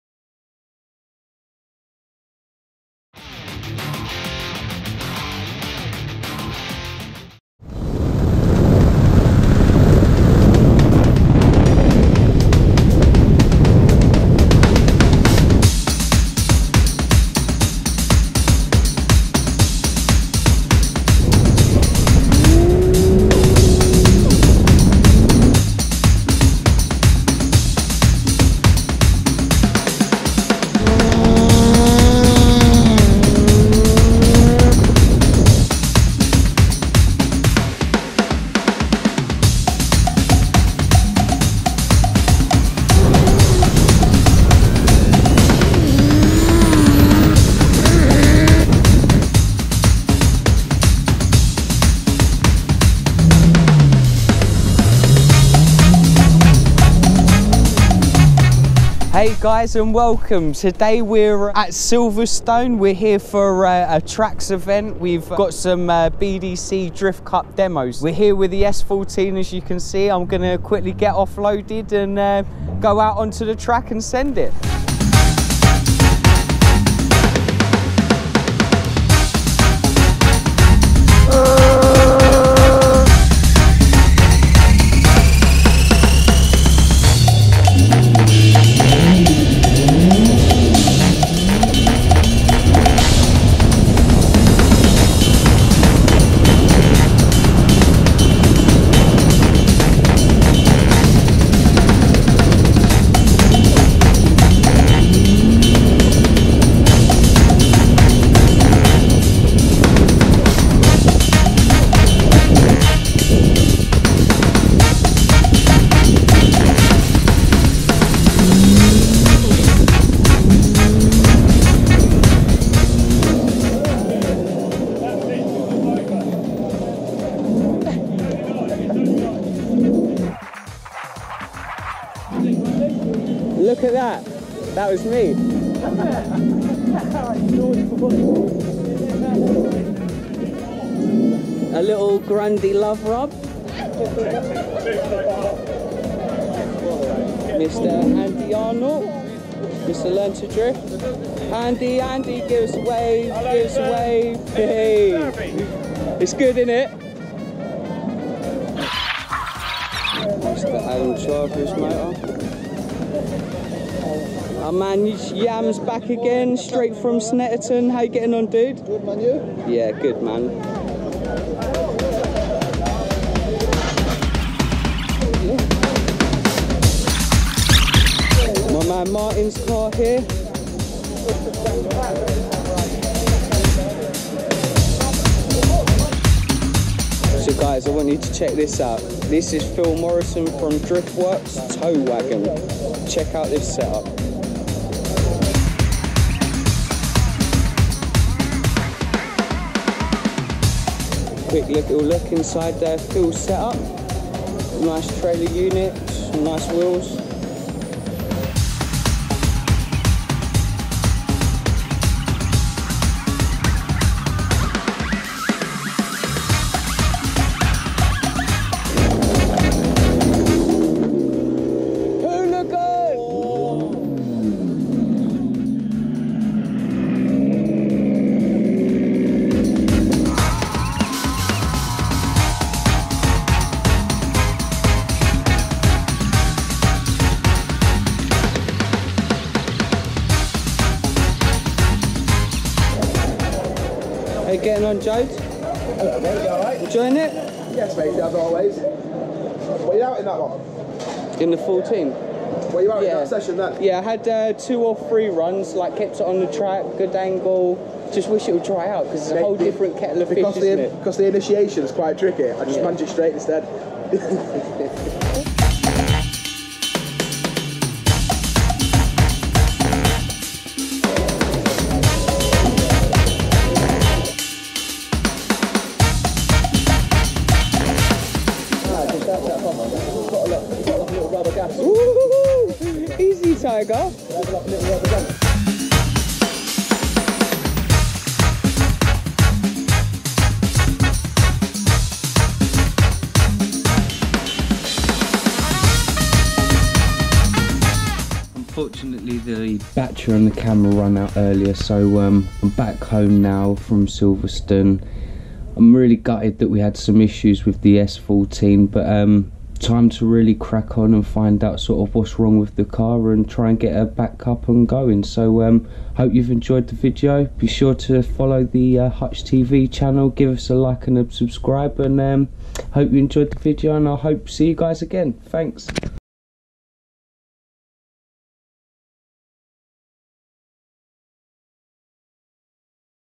Drifting with the best at Silverstone TRAX
TRAX, the ultimate performance car event! So many great cars and a lot of great drivers. Silverstone never fails to deliver.